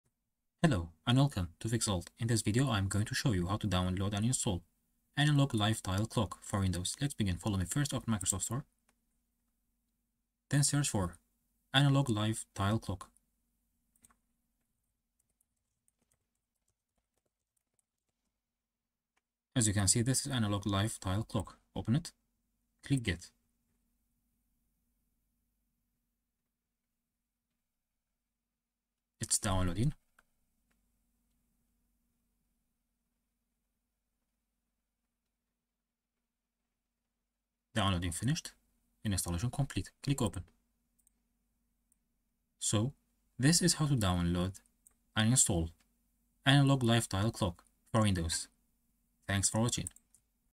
How to Download and Install Analog Live Tile Clock For Windows
Analog Clock on your Start screen.

This app enables you to add an analog clock on Start screen. You can select some options of clock design. This is "Analog" version of "Digital Live Tile Clock".

How to use:
Step 1. Plese pin this app to start manu.
Step 2. Customize tile design. Preview image will be changed.
Step 3. Press [Set Tile] to generate tile images and schedule live tiles.
Step 4. (optional) Right-click or click right-bottom button to pin additional tiles (up to 3).

- Due to limitation of live tile, 'seconds' cannot be displayed.
- Due to limitation of file size, complex background image cannot be used.
- Approx. 10 to 50 MB will be required to store image files on your local storage.
- On some environment, live tile may not work well.